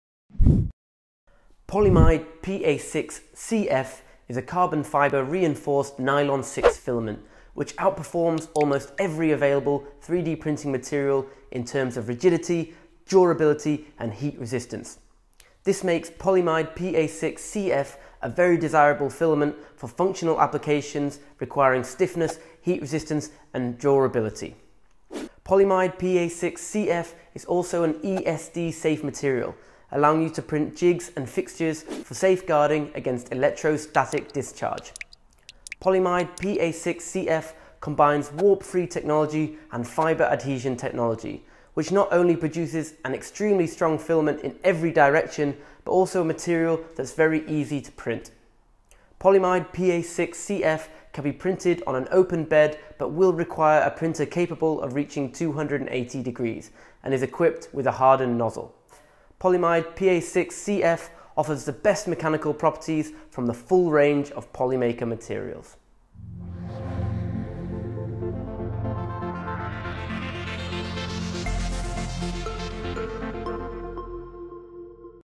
Filament PolyMide PA6 CF Polymaker : grande résistance thermique
Découvrez toutes les caractéristiques et applications possibles du PolyMide PA6 CF Polymaker. Ce nylon renforcé en fibre de carbone possède une excellence résistances aux impacts ainsi qu'aux déflexions thermiques. A découvrir sur makershop.fr